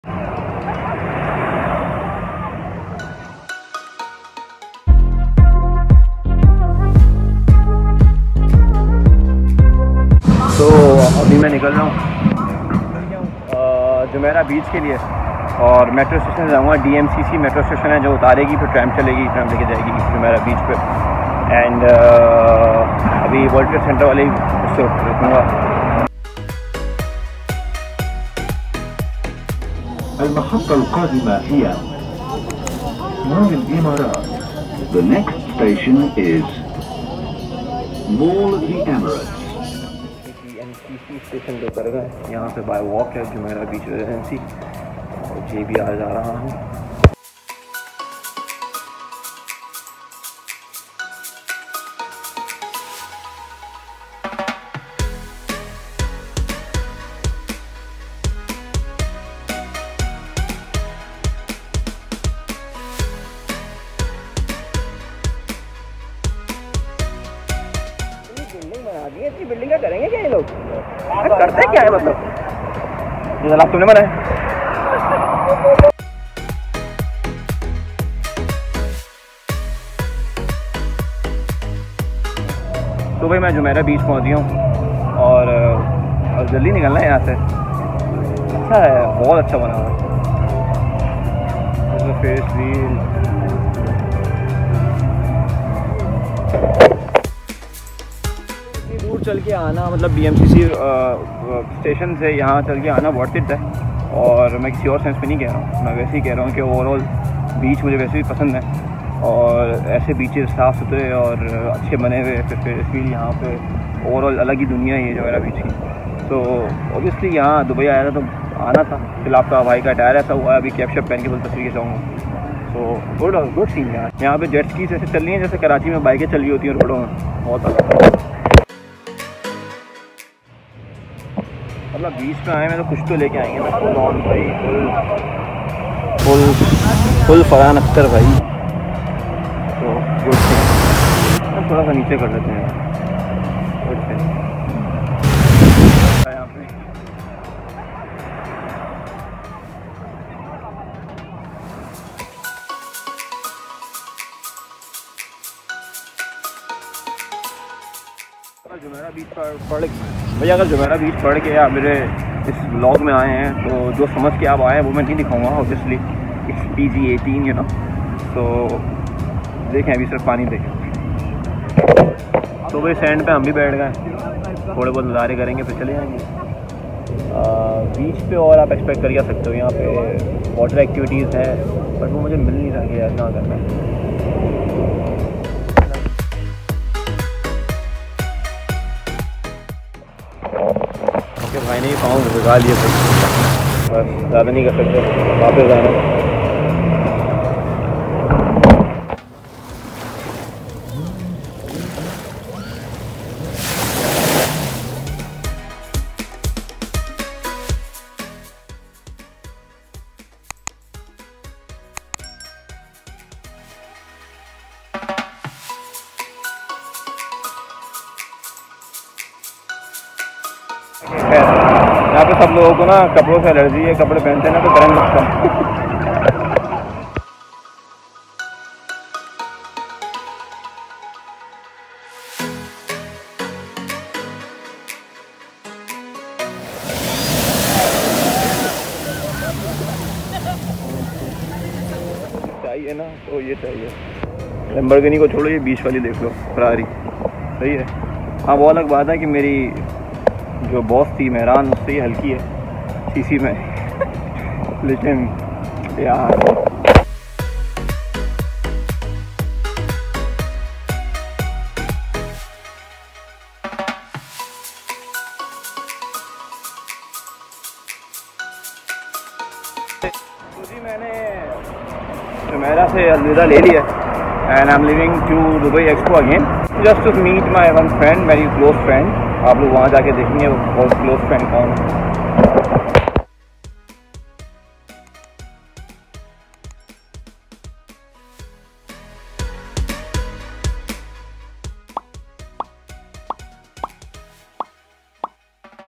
JUMEIRAH BEACH DUBAI | VLOG | DUBAI DIARIES
So i visited dubai in March 2022 and jumeirah beach to visit banta tha aur wahan ke haseenaye ;)

Thanks to @VlogNoCopyrightMusic for the background score